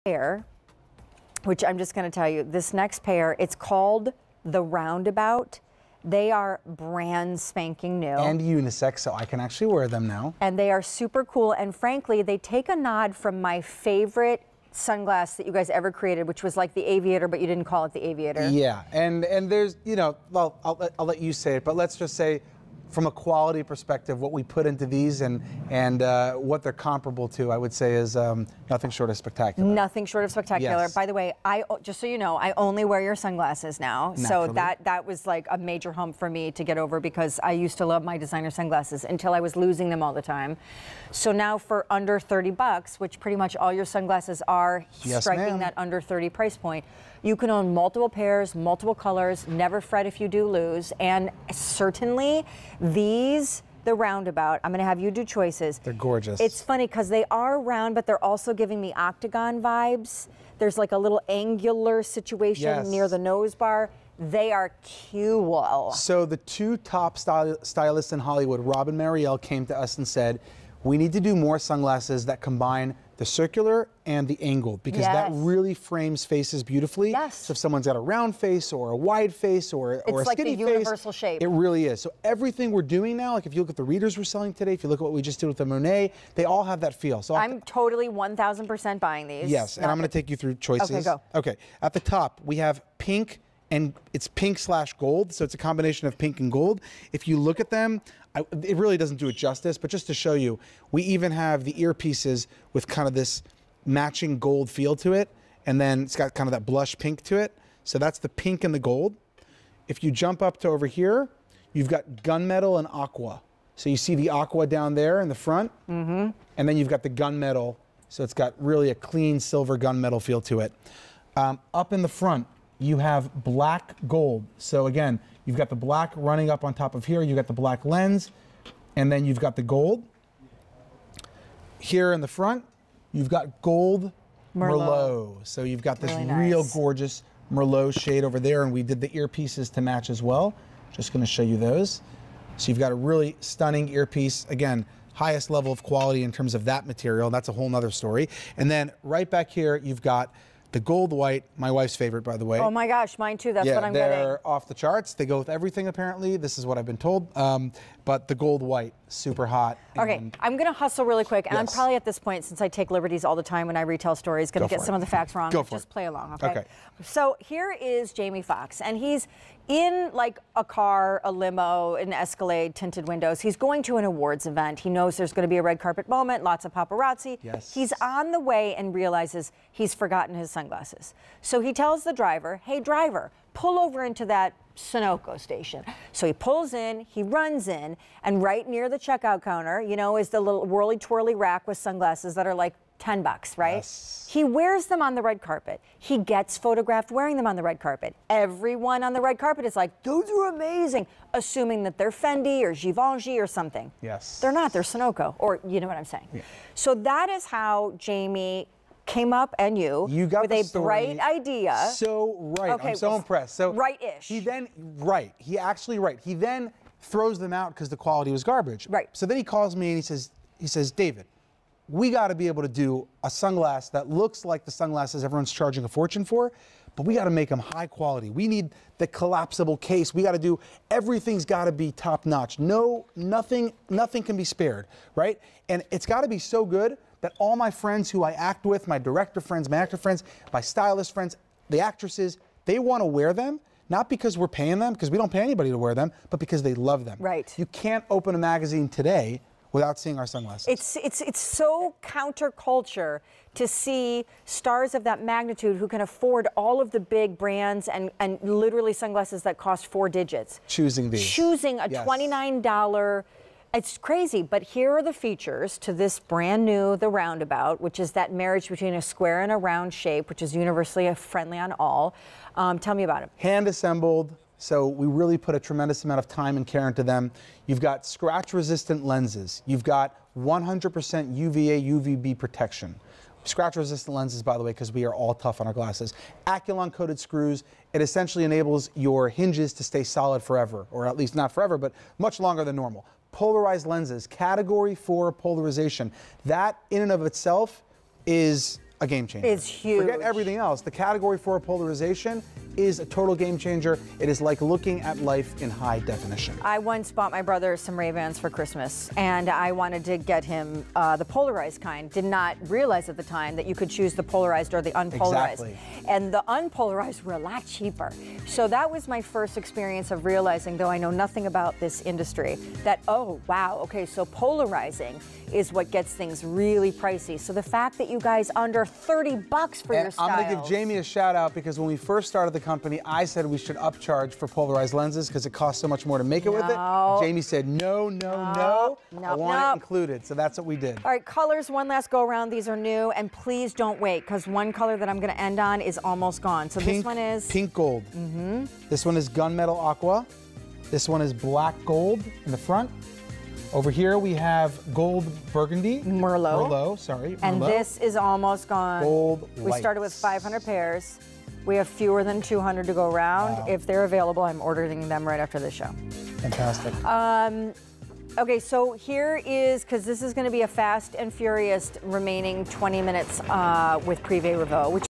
Prive Revaux Roundabout Polarized Sunglasses on QVC
Prive Revaux Roundabout Polarized Sunglasses

Even if you've come by these polarized sunglasses in a roundabout way, they'll take you straight through spring, summer, and any sunny day in style. From Prive Revaux.